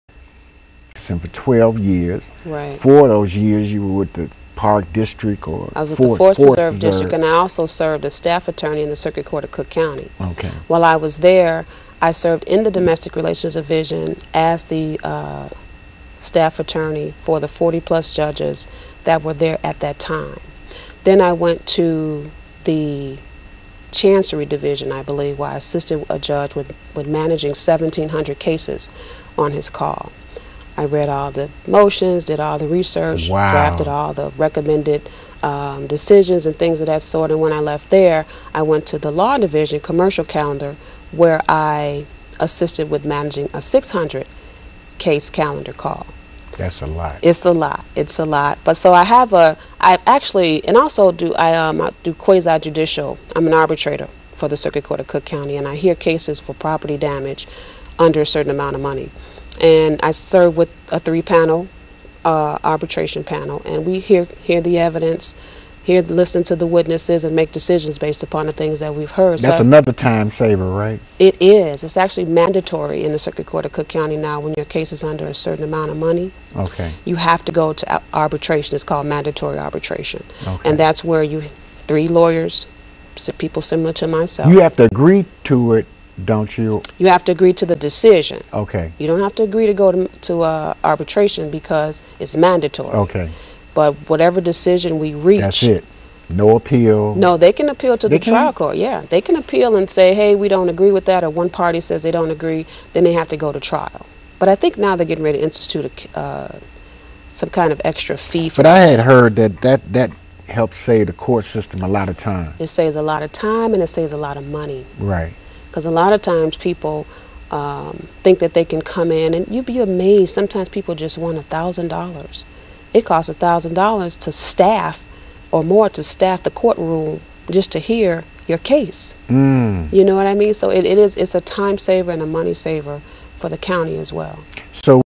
Rhonda Sallee more on her background and Education
Rhonda is a Candidate for Judge of the Circuit Court of Cook County,